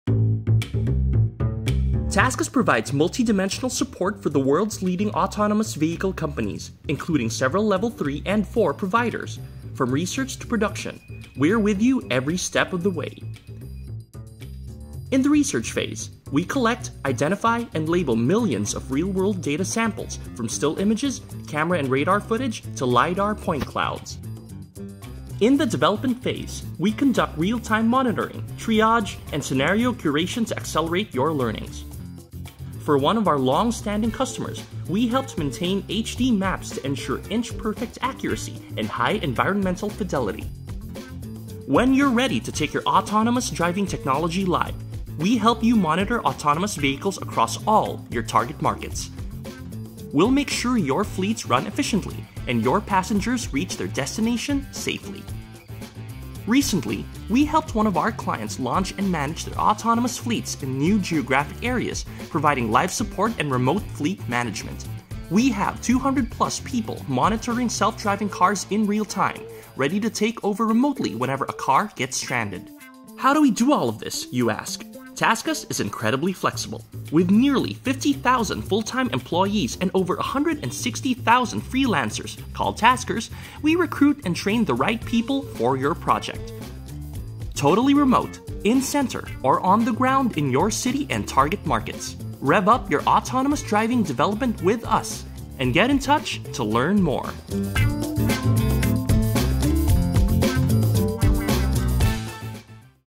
Driving Innovation Together: TaskUs - Your Trusted Partner in Autonomous Vehicles
In this video, learn how TaskUs collaborates with top-tier Level 3 and 4 providers to accelerate the future of self-driving cars. From groundbreaking research and development to seamless production, TaskUs supports every stage of the AV revolution. Our dedicated team of experts works hand-in-hand with industry leaders, providing comprehensive support throughout the entire autonomous vehicle lifecycle. See TaskUs in action, driving the future of autonomous vehicles.